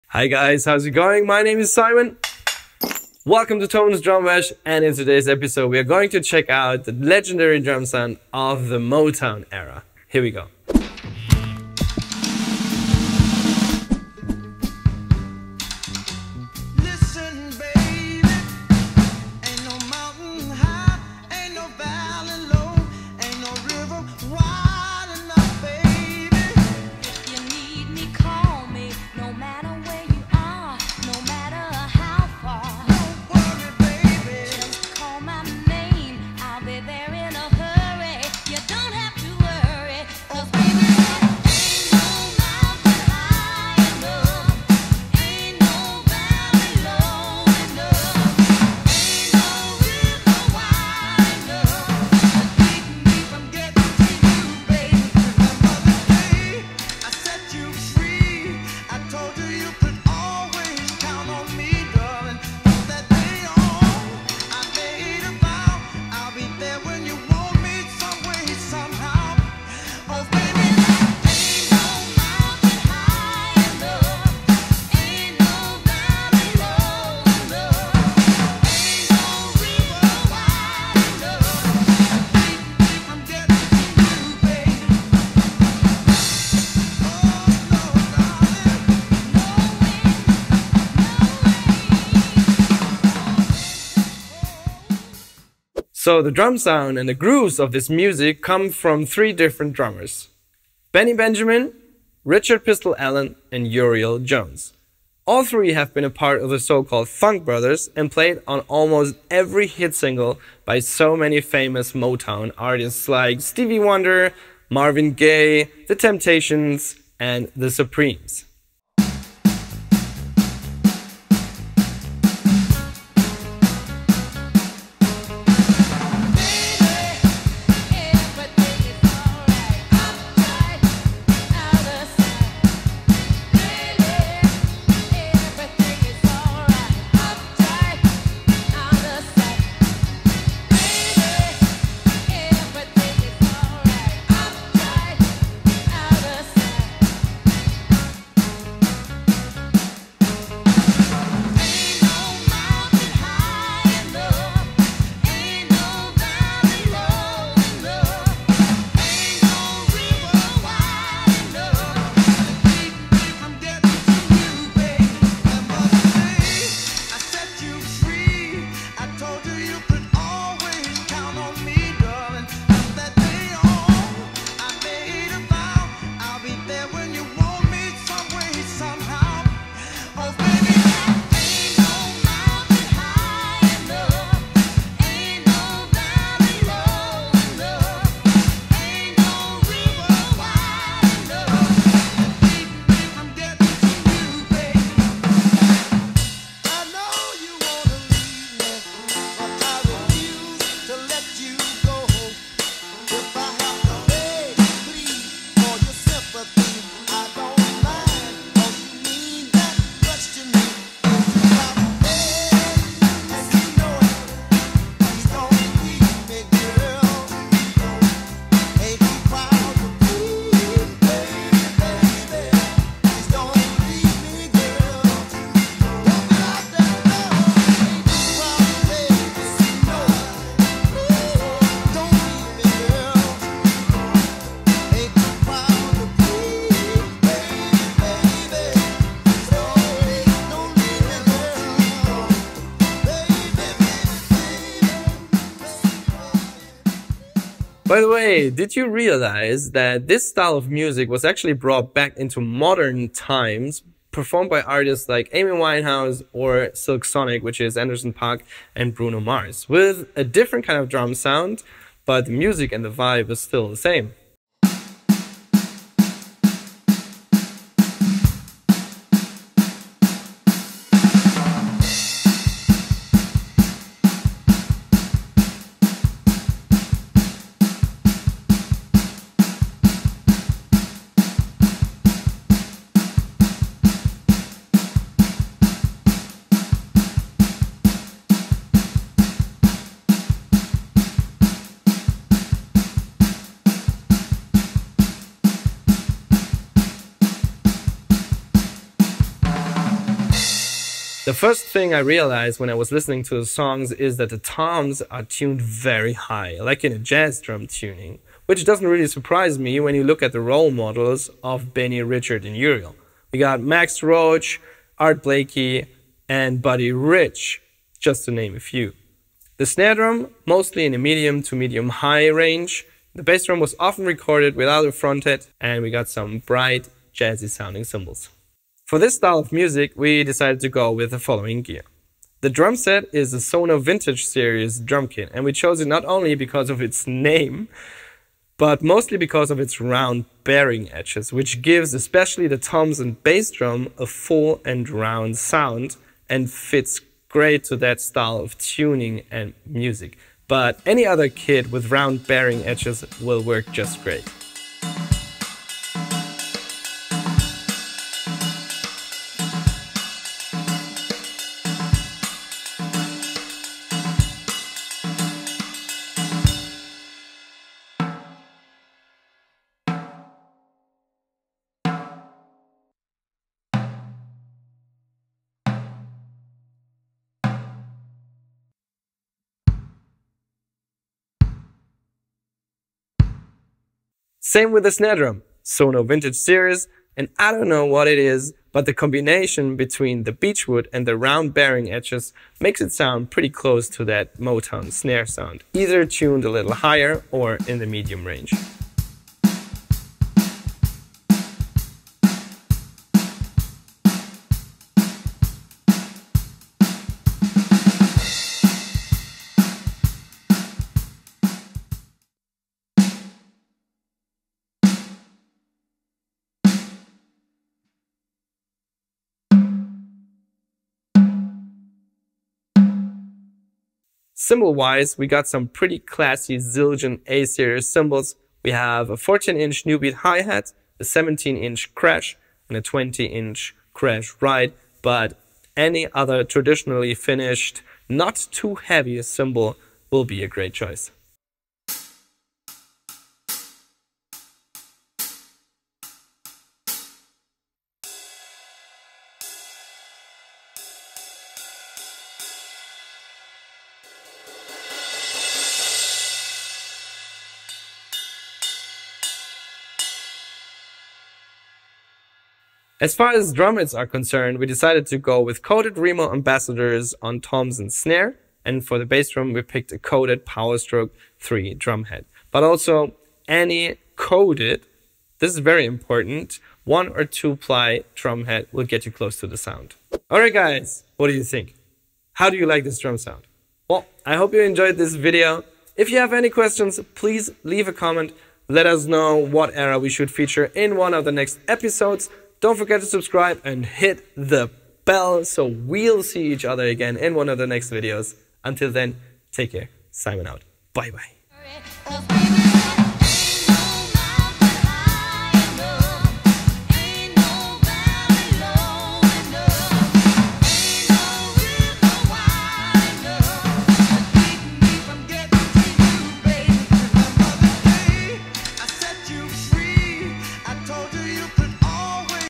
The Sound of Motown Drumming | Drum Lesson | Sounds of an Era | Thomann
The drum sound of Motown genre just is legendary - 3 drummers mainly created the unique sound of the band: Benny Benjamin, Uriel Jones, Richard "Pistol" Allen. In today's episode, Simon shows you how to re-create the drum sound and which setup is ideal for the Motown sound.

Interesting bits and pieces:
0:00 - Welcome back
0:11 - Let's listen to the drums
1:35 - The drummers of the song
2:03 - Some more song inspo
4:07 - Bringing the sound back to modern times
5:01 - The tuning
5:29 - The gear
8:00 - How do you like the drum sound?